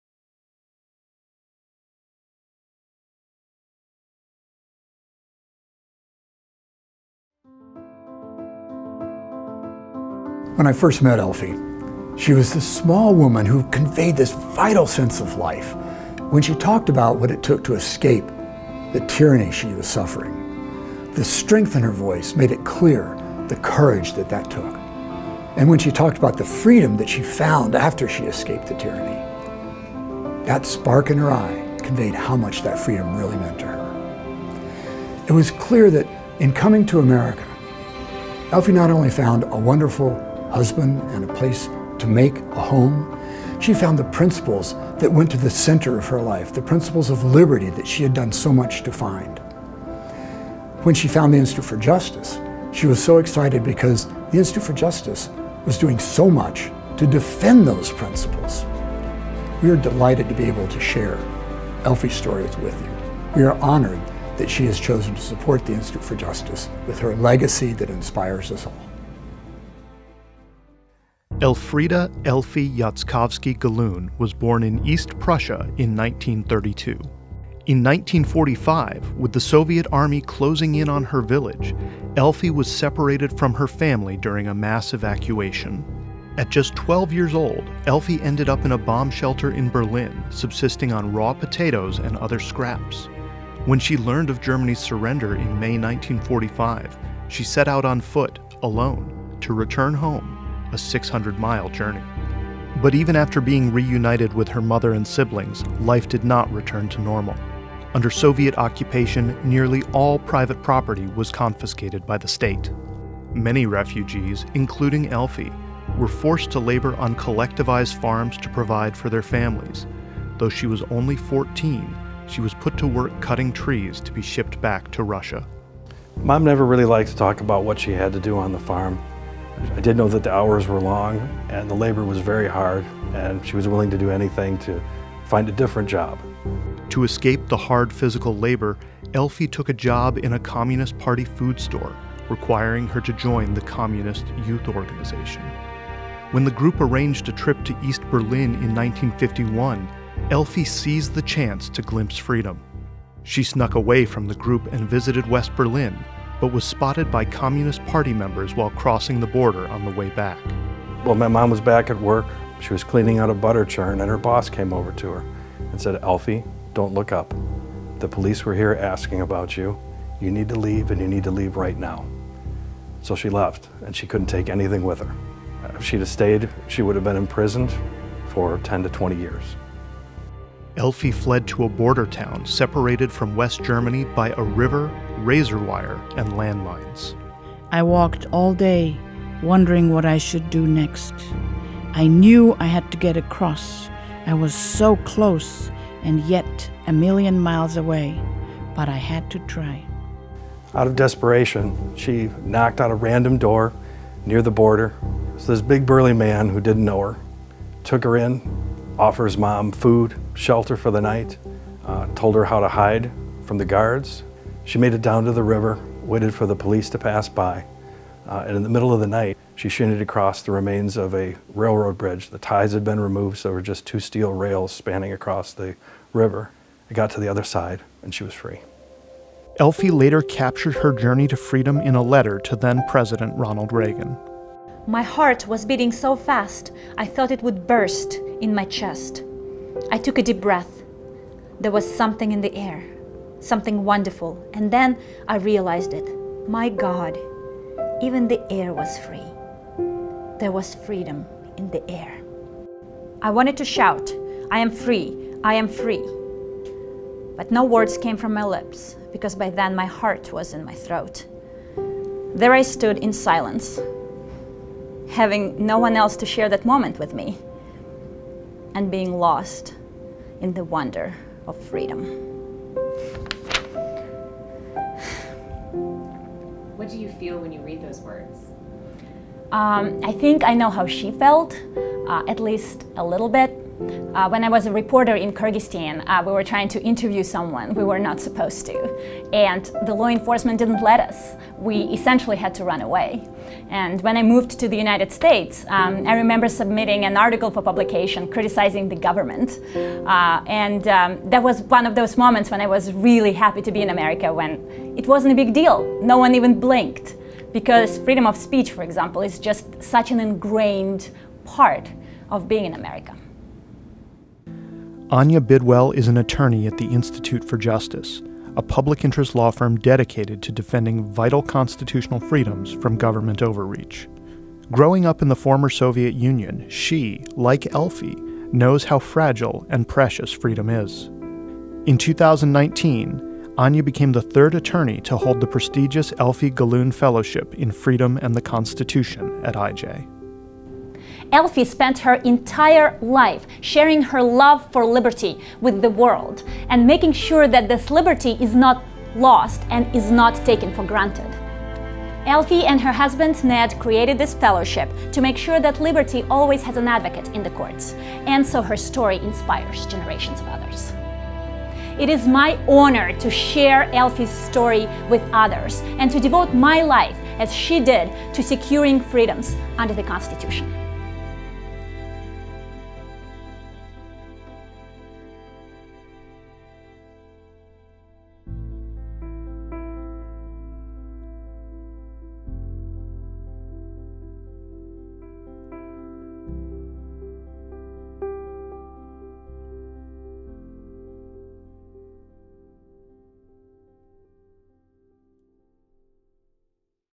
Journey to Freedom: Elfie Gallun’s Incredible Escape to America
Elfie Gallun fled communist East Germany for the promise of freedom. Her legacy lives on at the Institute for Justice.